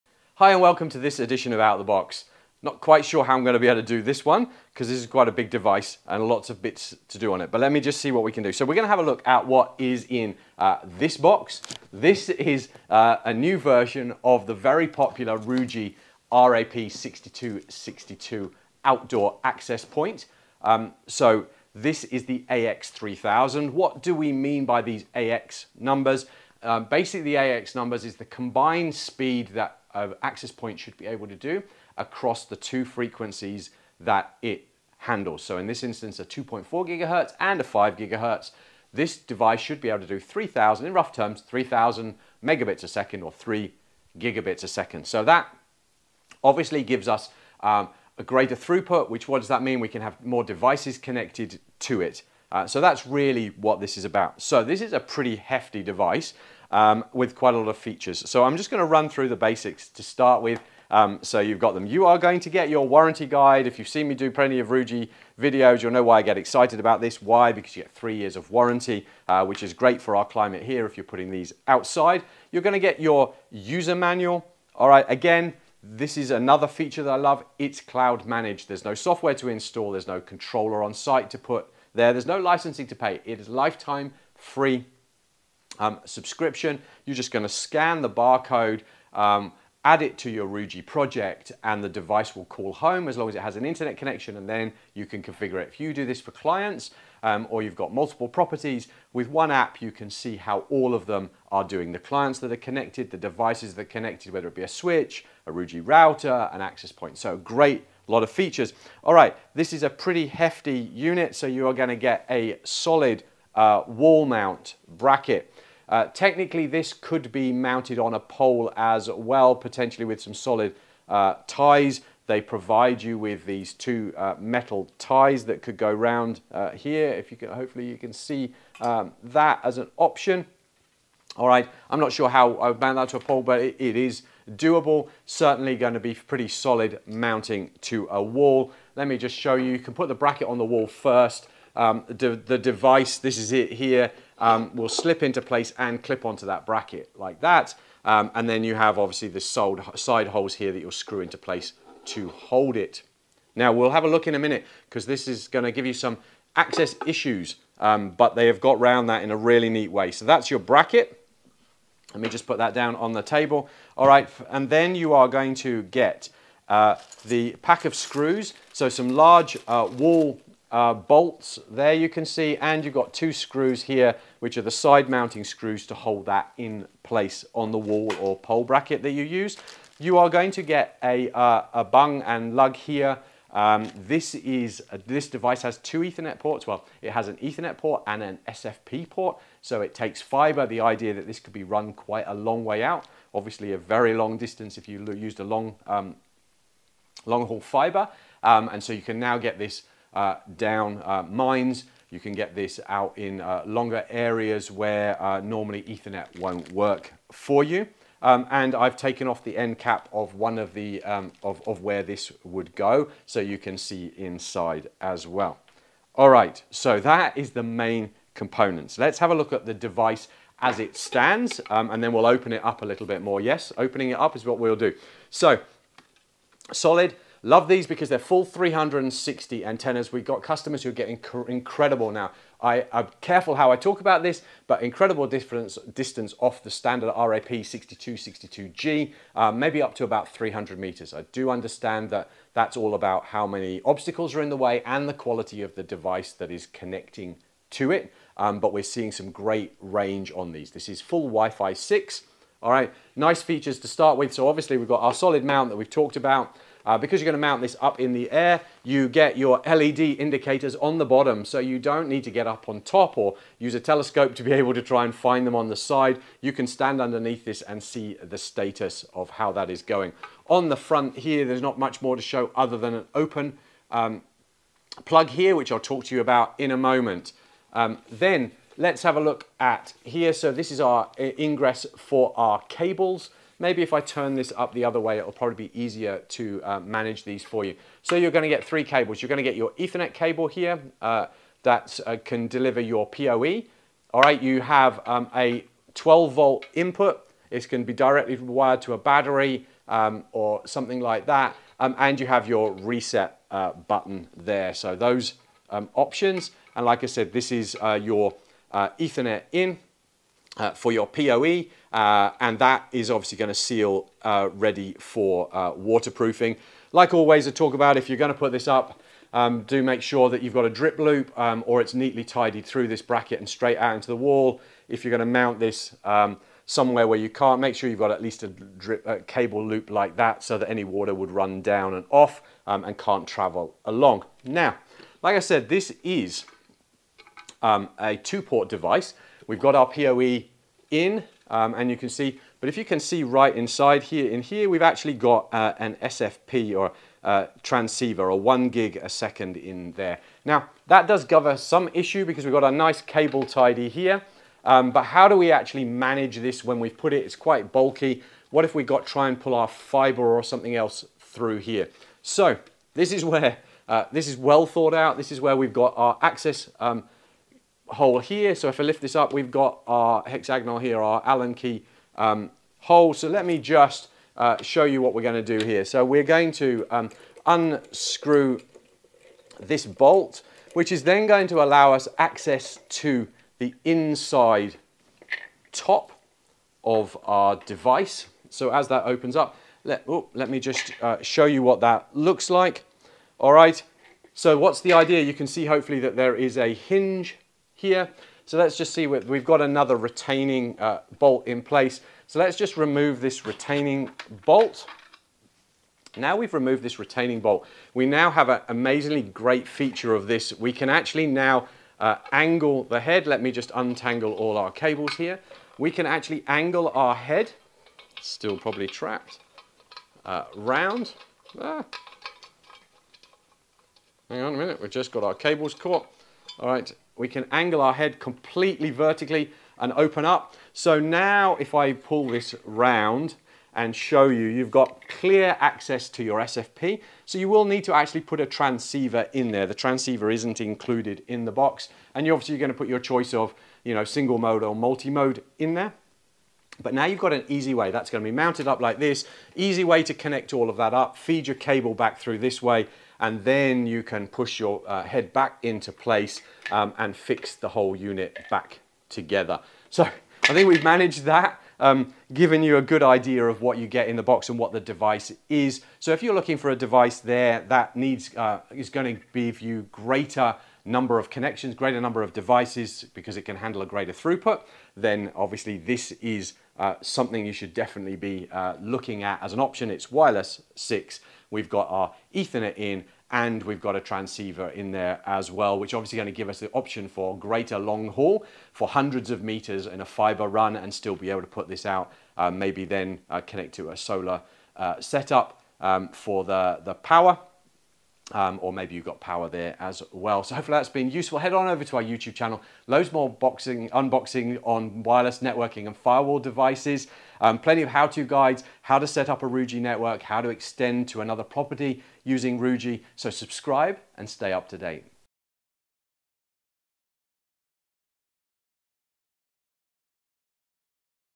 Out the Box Series - Ruijie RG-RAP6262 AX3000
This short few minute video, gives you a quick overview of what you can find in the box when you receive your Ruijie RG-RAP6262 AX3000.

Enter the Era of Ultra-high Wi-Fi 6 Speed: With a revolutionary combination of OFDMA and 1024QAM, the latest 802. 11ax standard, and the 160MHz band, RG-RAP6262 wireless aggregation rate reaches 2.976Gbps.
With gigabit SFP port, the device supports up to 20km transmission from datacentre with single mode fibre.

Rock-solid in Harsh Environments: Adopts completely sealed, waterproof, dustproof, moisture proof and flame-retardant, wind erosion protection, wide temperature range, and 4kV surge protection design.

Omni-directional Coverage with Long Range: Built-in 5 high gain antennas, supports full coverage with radius of 100 meters at 2.4GHz, and 300 meters at 5GHz

Easily Expand Extra Outdoor Wi-Fi with Reyee Mesh: Less wiring, more relaxing

Easy Installation, Firmly Fixed: Rack-separated mounting design, easily installation on high. Replace plastic cable ties by iron hoop.

Please note - You need a PoE Injector to power this or for it to be connected to a PoE switch.

Chapters
00:00 Intro
00:54 What's in the box?
01:49 Wall mounting
03:36 The device
06:08 Access hole
07:40 Outro